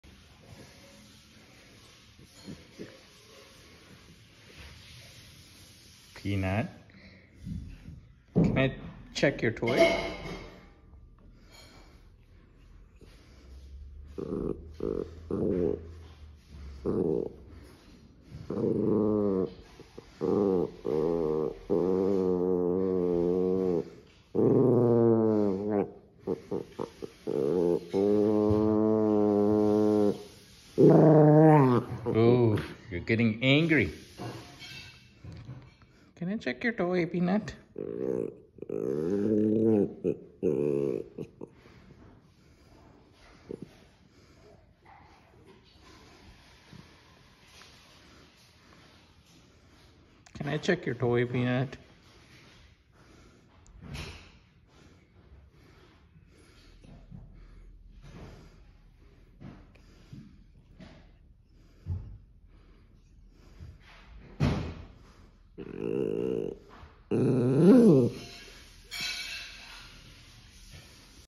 Maltipoo gets angry if I touch his toy
Maltipoo growls when I touch his toy